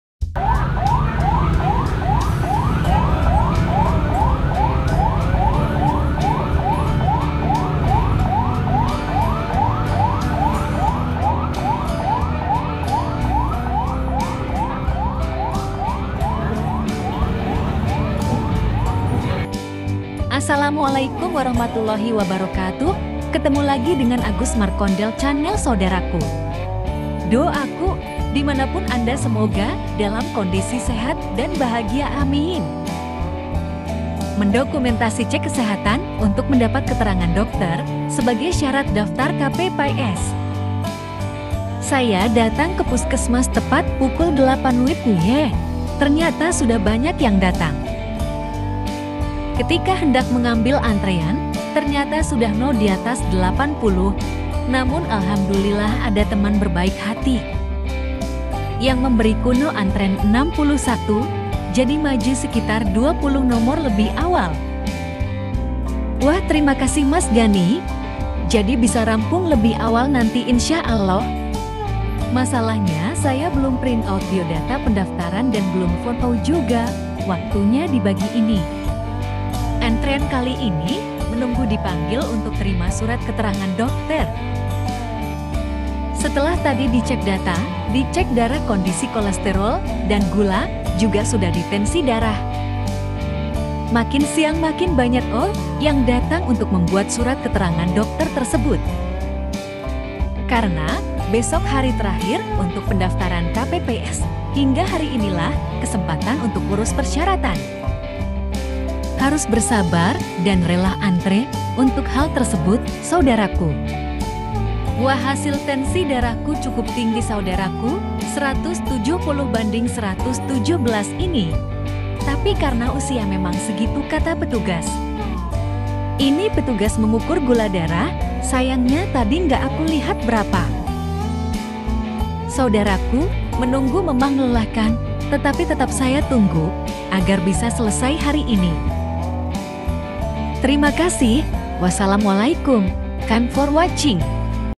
PUSKESMAS RAWALO BANJIR WARGA ♥️♥️ MASYARAKAT RAWALO BIKIN KET. DOKTER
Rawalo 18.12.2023

Puskesmas diseluruh Indonesia, dipadati pengunjung untuk membuat surat keterangan dokter yang menyatakan sehat.
guna persyaratan pendaftaran anggauta KPPS .